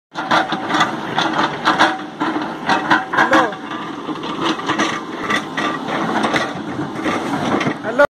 for btech civil students
making a new road before root up old pichu by jcb's scene